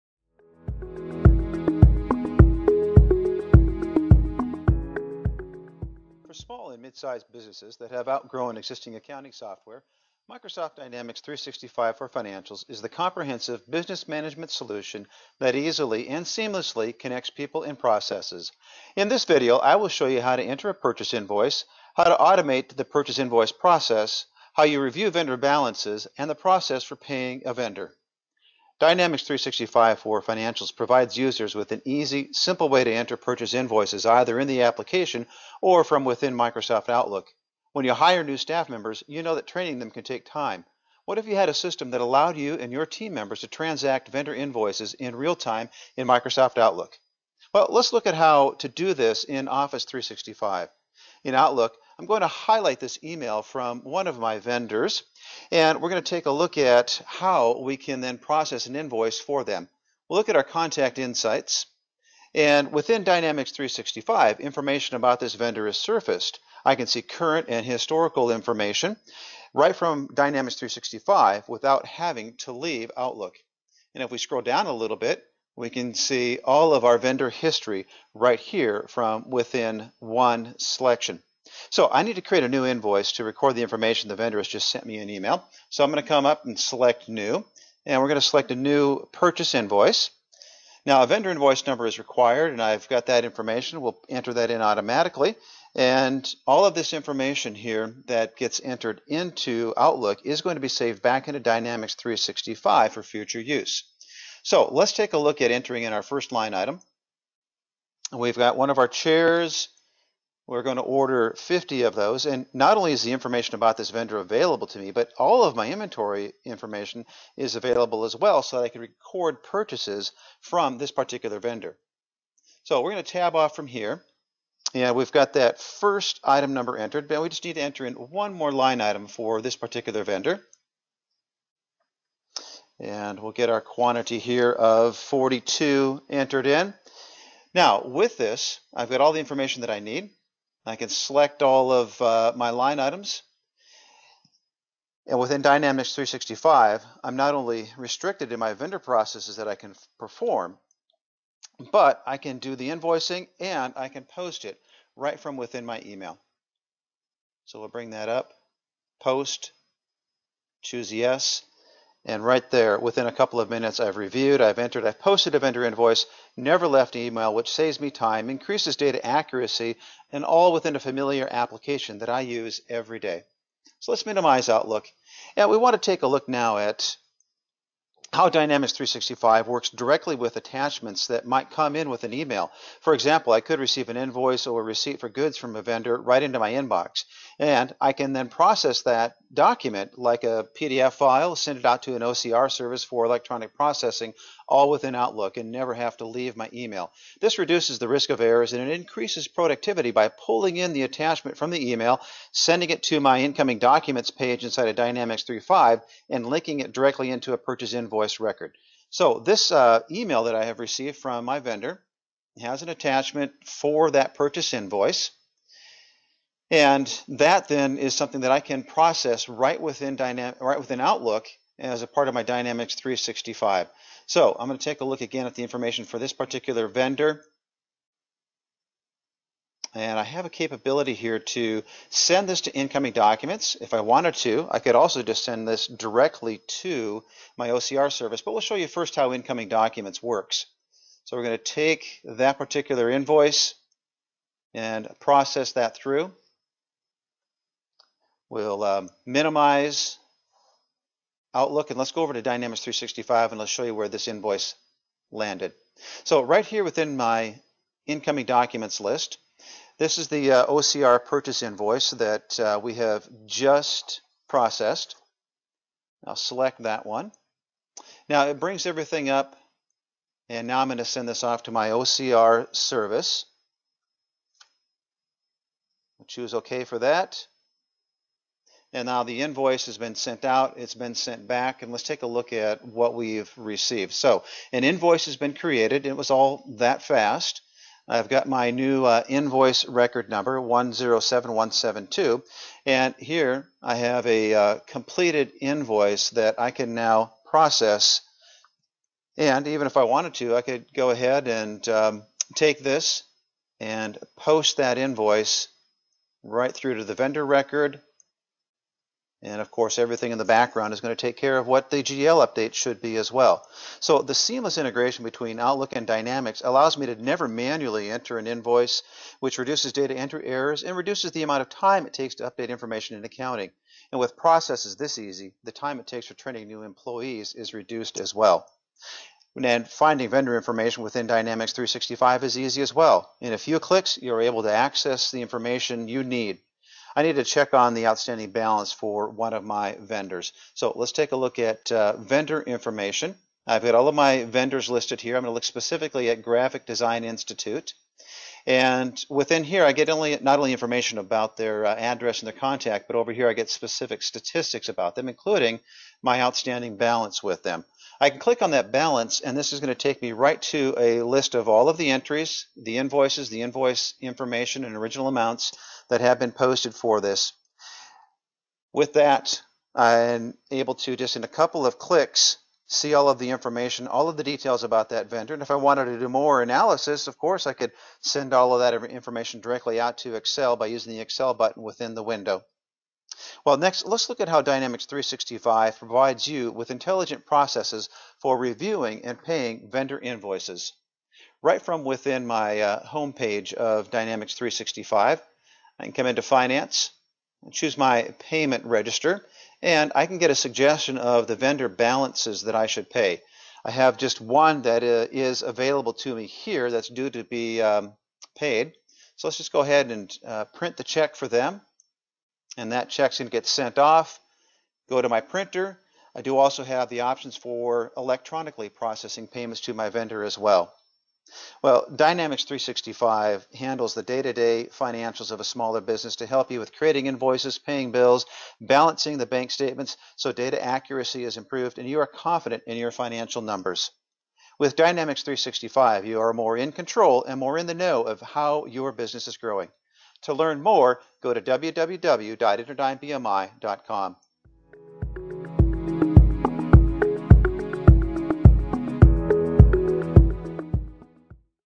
Microsoft Dynamics 365 — Bookkeeping Introduction
Mark Jensen of InterDyn BMI introduces the bookkeeping capabilities of Microsoft Dynamics 365.